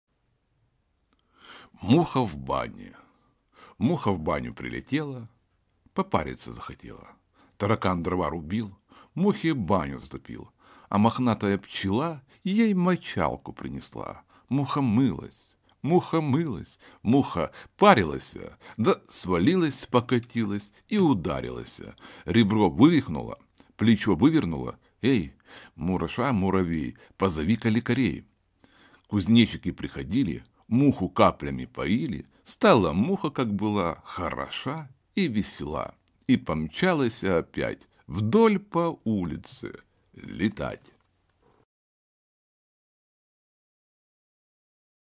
Муха в бане, Корней Чуковский, читает Павел Беседин
Муха в баню прилетела,
Попариться захотела.

Таракан дрова рубил,
Мухе баню затопил.

А мохнатая пчела
Ей мочалку принесла.

Муха мылась,
Муха мылась,
Муха парилася,
Да свалилась,
Покатилась
И ударилася.

Ребро вывихнула,
Плечо вывернула.

«Эй, мураша-муравей,
Позови-ка лекарей!»
Кузнечики приходили,
Муху каплями поили.

Стала муха, как была,
Хороша и весела.